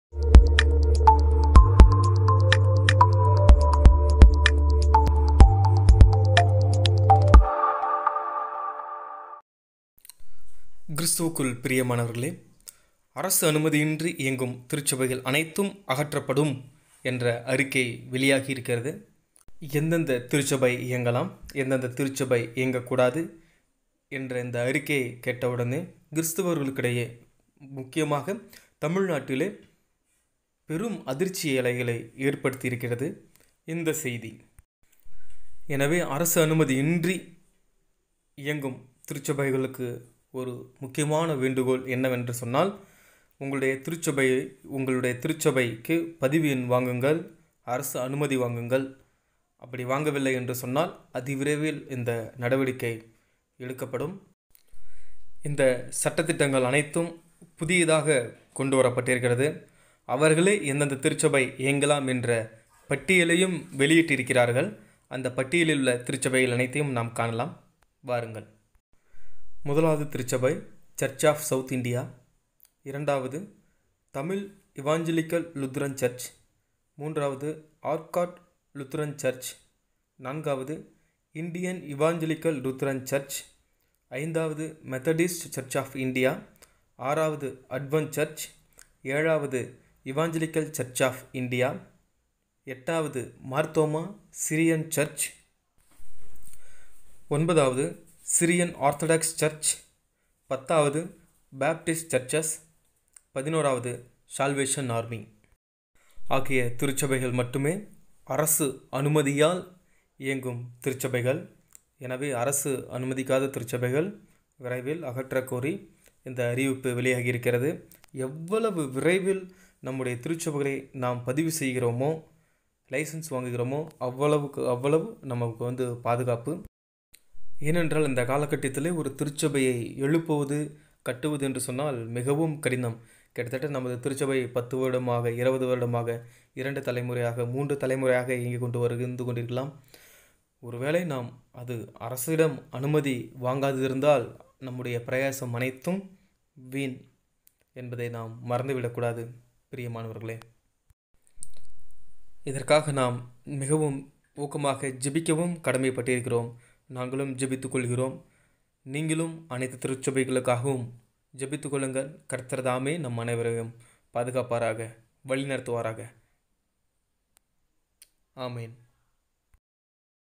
New Government order for Church License || திருச்சபைகள் நடத்துவதற்கும் அரசு அங்கீகாரம் வேண்டும்
இது நமது இந்திய அரசால் கொண்டுவரப்பட்ட சட்டம் ஆகவே கவனமாக செயல்பட வேண்டும் என்பது எங்கள் கோரிக்கையும் ஜெபமும்.

This new law is brought by our indian goverment therefore be aware in church issue. in case you don't have the licence for your church kindly get approval from our government . it's must. Don't forget. this is what our prayers and requests.

For Prayer Requests & info :- (24×7)

"JEHOVAH JIREH MINISTRIES"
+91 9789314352